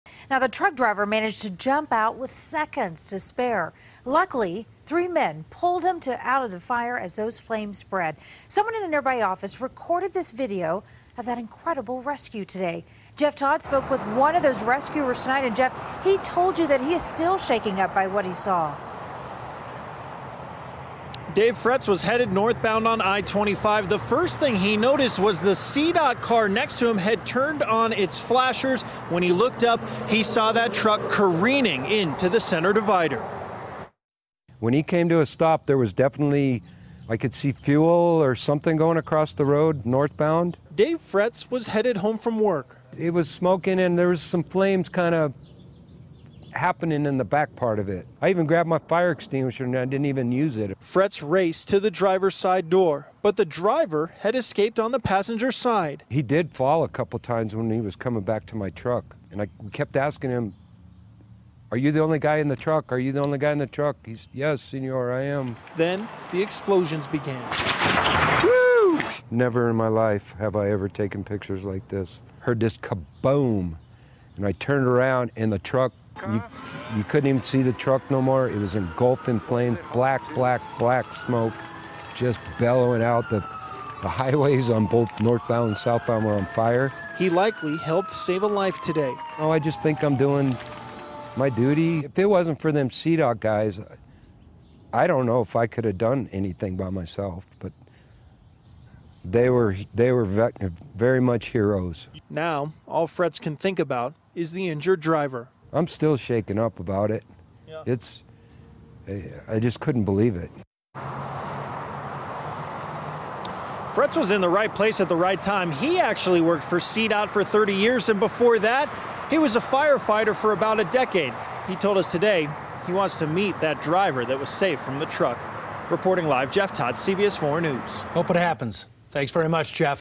'I Heard This Kaboom!': Witness Helps Driver From Burning Semi
The driver of the burning semi truck in the Denver Tech Center escaped from the inferno thanks to the quick thinking of some witnesses.